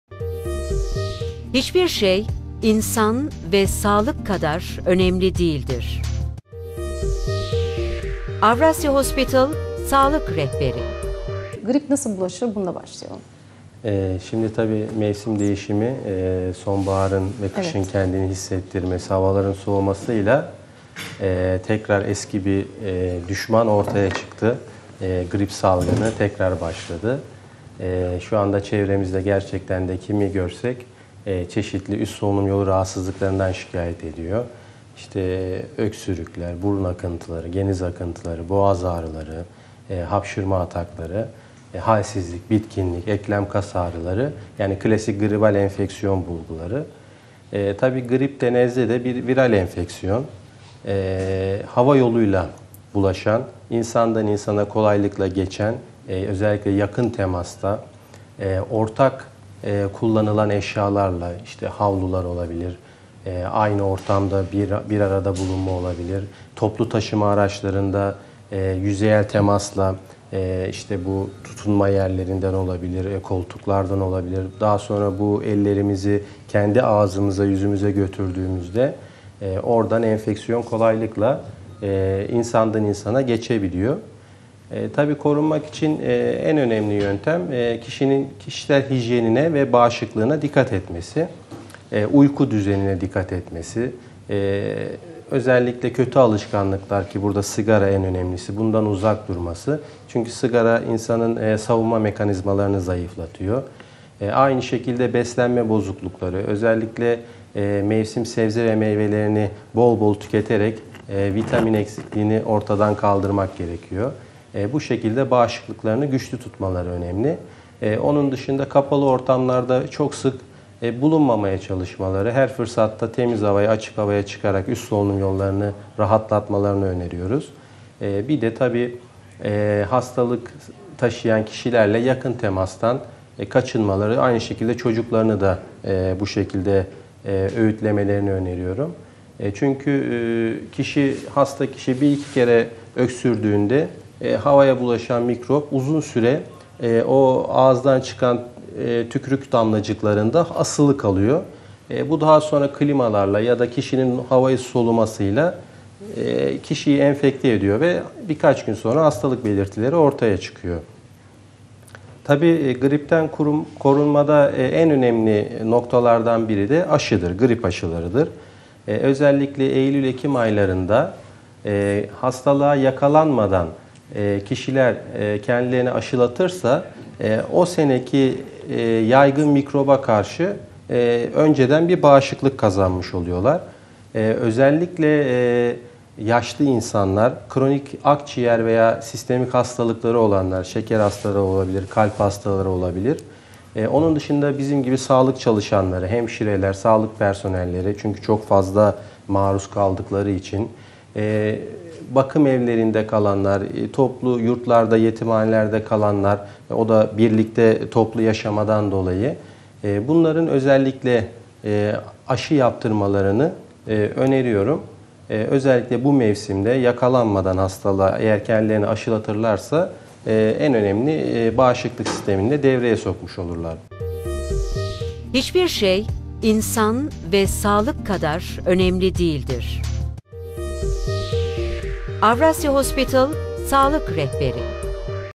Avrasya Hospital
Grip Nasıl Bulaşır ? Avrasya Hospital Kulak Burun Boğaz Uzmanı Op. Dr. Galip Arda Pelen Bilgilendiriyor.